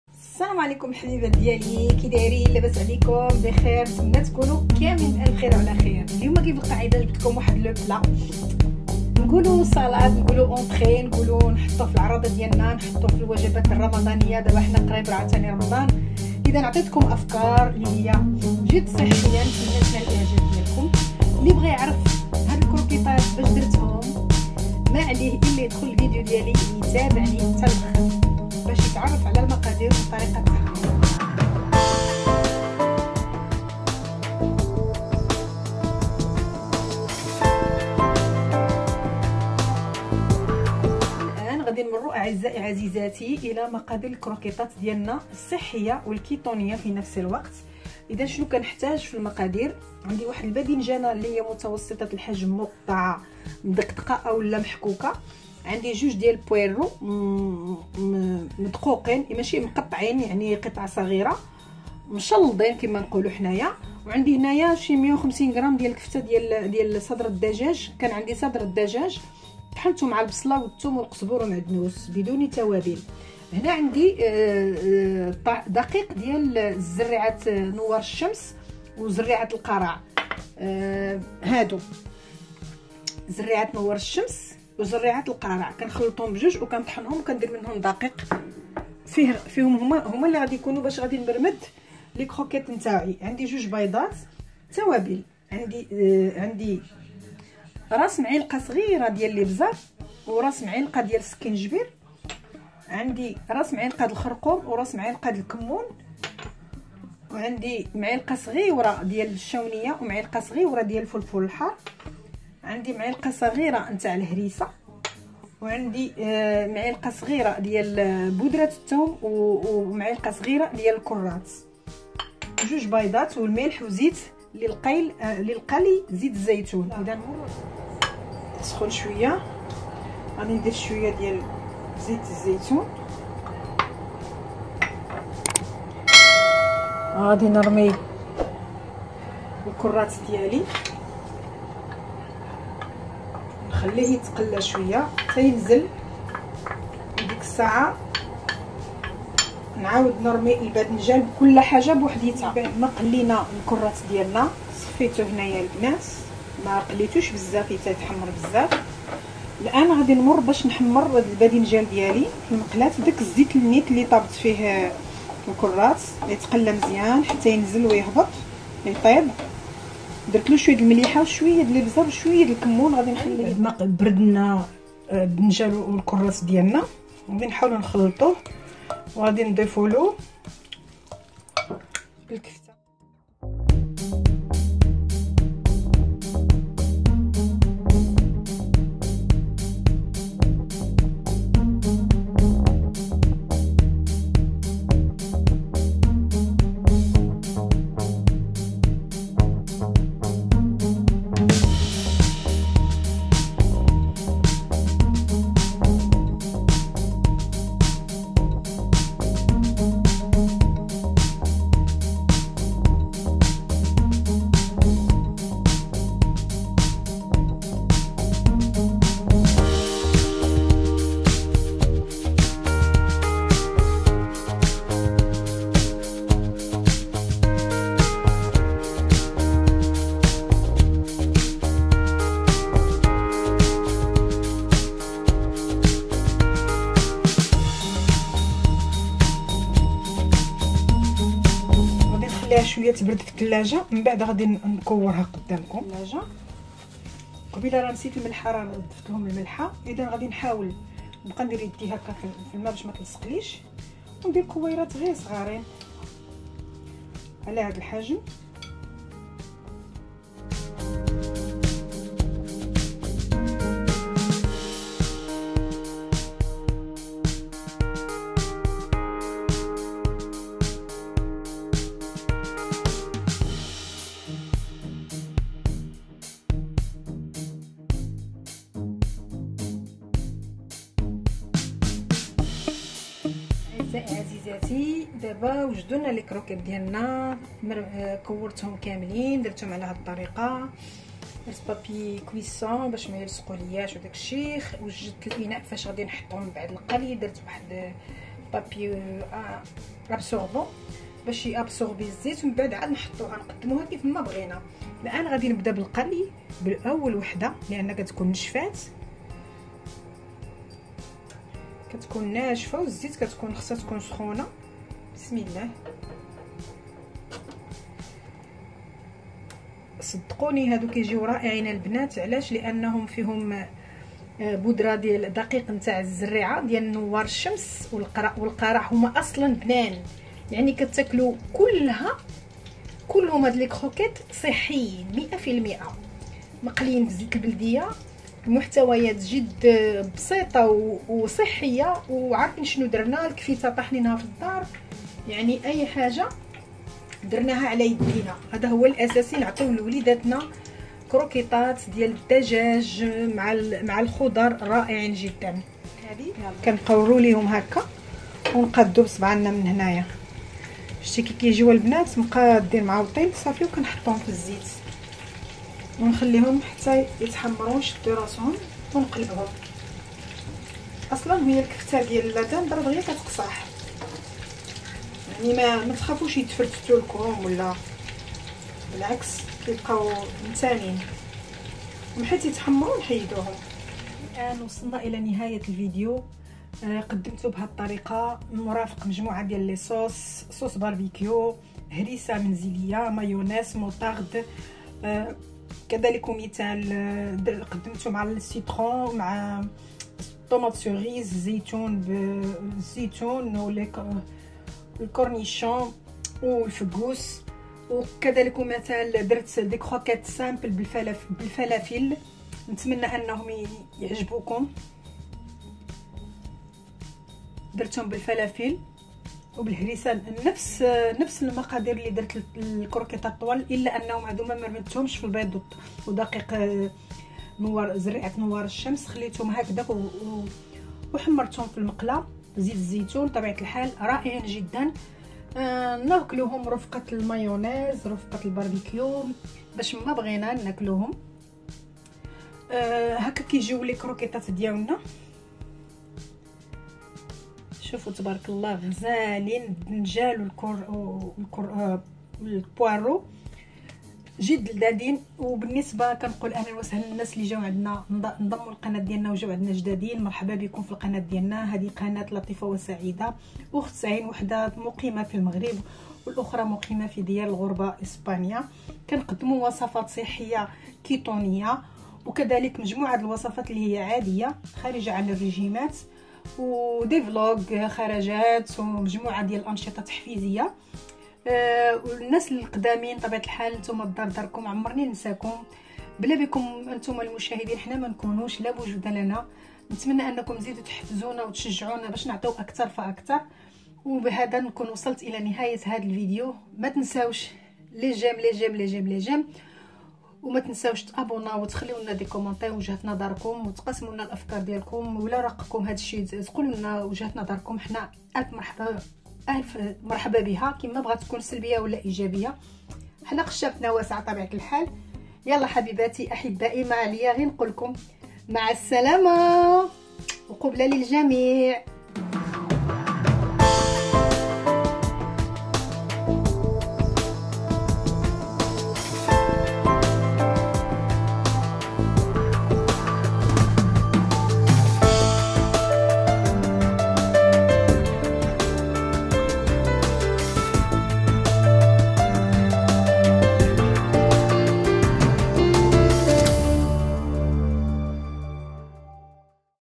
الى دقتيهم ما بقيش تستغناي عليهم ،كروكيطات الباذنجان و صدرالدجاج صحية وكيتونية
المقادير : بادنجال متوسطة الحجم مشلدة او مقطعة الى مربعات صغيرة / اثنين بويرو مقطع قطع صغيرة ايضا / 100
الى 300 كغتة صدر الدجاج او الديك الرومي مطحون بالبصل والثوم وقزبرة وبقدونس /  بيضتان / دقيق بذور نوار الشمس وبذور القرع الاحمر / زيت الزيتون للقلي او زبدة حسب الرغبة / ملح .
التوابل :  راس ملعقة صغيرة كمون / فلفل اسود / راس ملعقة خرقوم / ملعقة صغيرة فلفل حار / ملعقة هريسة / ملعقة صغيرة بودرة الثوم / ملعقة صغيرة كرات/ ملعقة فلفل احمر حلو